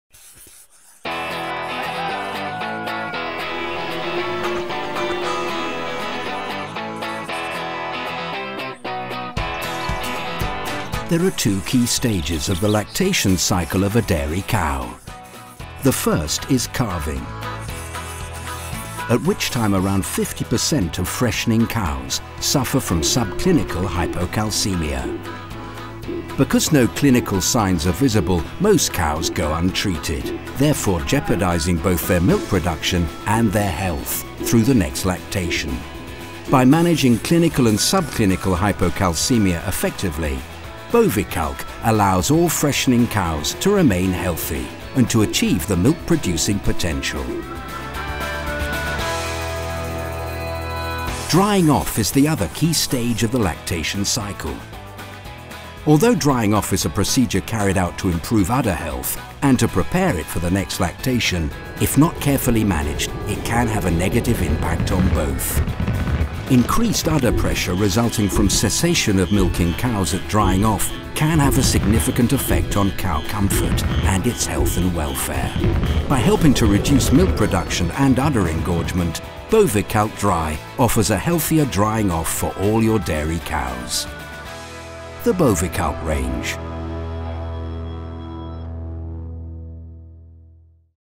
Managing the most important stages of the lactation cycle
There are two key stages of the lactation cycle of a dairy cow. the first is calving at which time around 50% of freshening cows suffer from subclinical hypocalcemia. Drying-off is the other key stage of the lactation cycle.